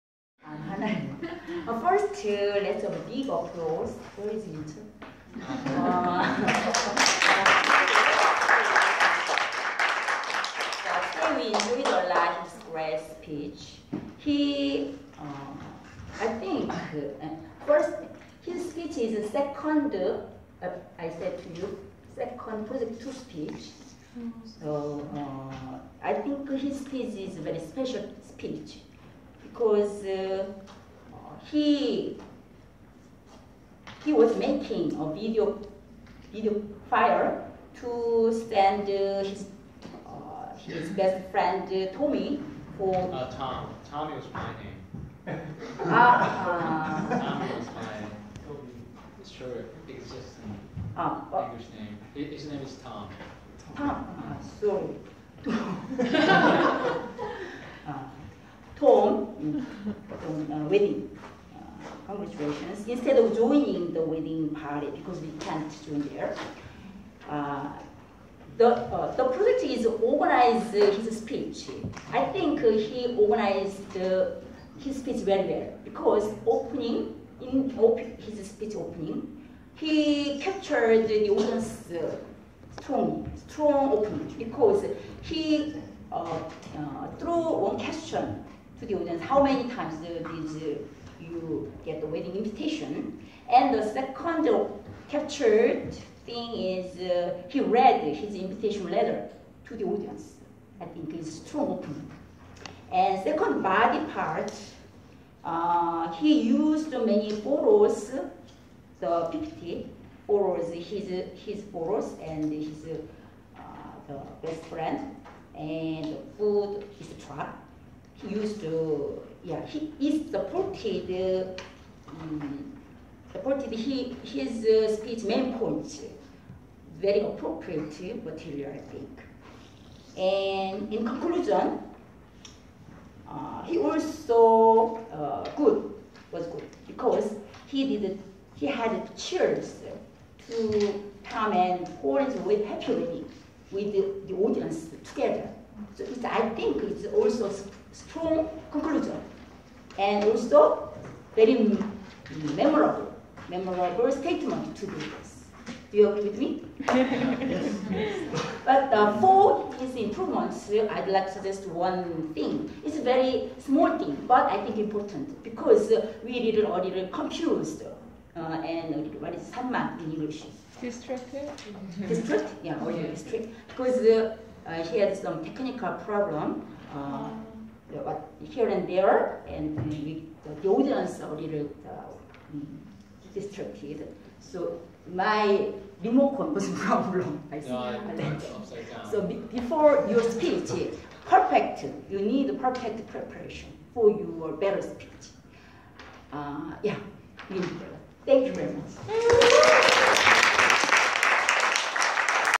20151016- YTM445th- Hana's Evaluation- Minchul's CC#2 Speech- 'To Tom and Pauline’s happy Wedding'
20151016- YTM445th(Yeouido Toastmaster Club)- Hana Lee's Evaluation- Minchul Jeong's CC#2 Speech(2nd)- 'To Tom and Pauline’s happy Wedding'

(20151016-YTM445th--Minchul Jeong's CC#2 Speech-'To Tom and Pauline’s happy Wedding' )